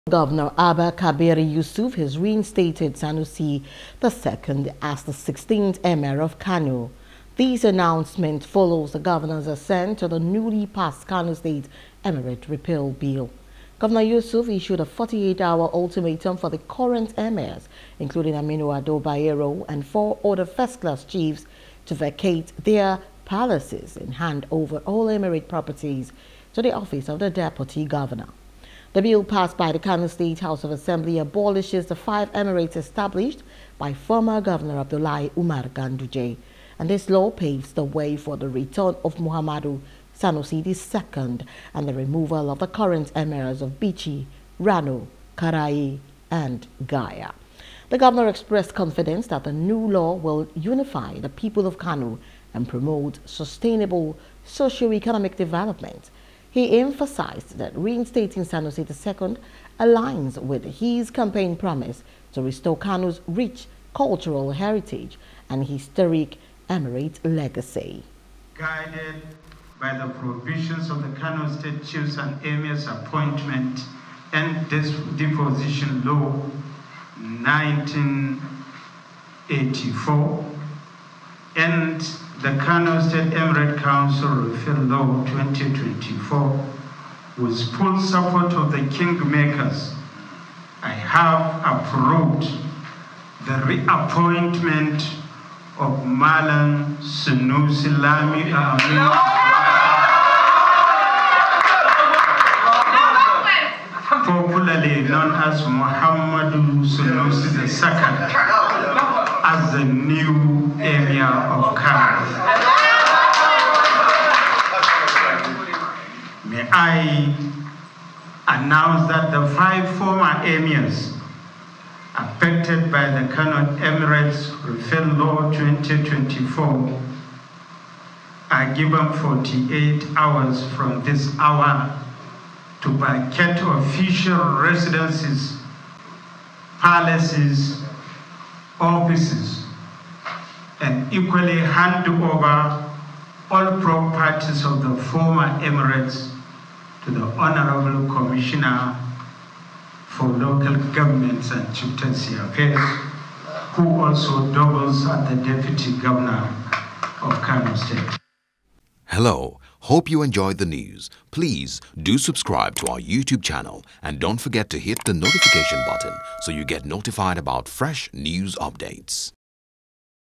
Emirate Law: Governor Yusuf Reinstates Sanusi Lamido As 16th Emir Of Kano
Governor Abba Kabir Yusuf has reinstated Sanusi II as the 16th Emir of Kano.